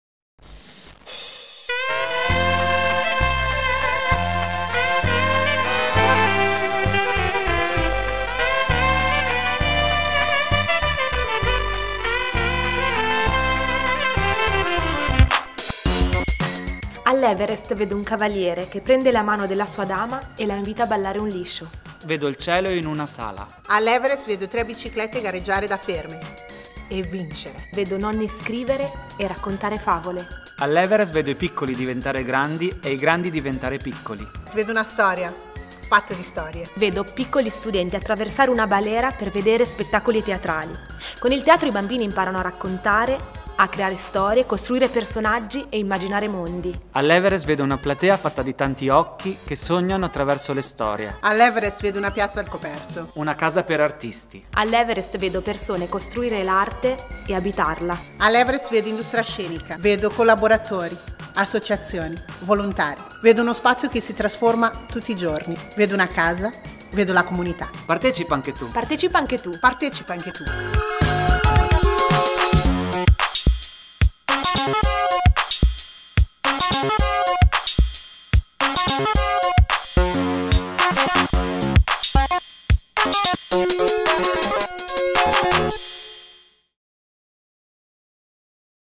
E Tu Cosa Vedi? Anche i piccoli vogliono vederci bene
Fra i numerosi traguardi che Industria Scenica vorrebbe raggiungere quest'anno, c'è quello per cui abbiamo pensato di attivare la raccolta fondi "E Tu Cosa Vedi?". Ospitando numerosi spettacoli teatrali, scelti e pensati per il progetto "teatro-scuola", desideriamo che tutti i bambini possano avere la stessa visione di uno spettacolo.
Sostenendo la nostra campagna alimenterai un fondo che verrà utilizzato per costruire una tribunetta di legno.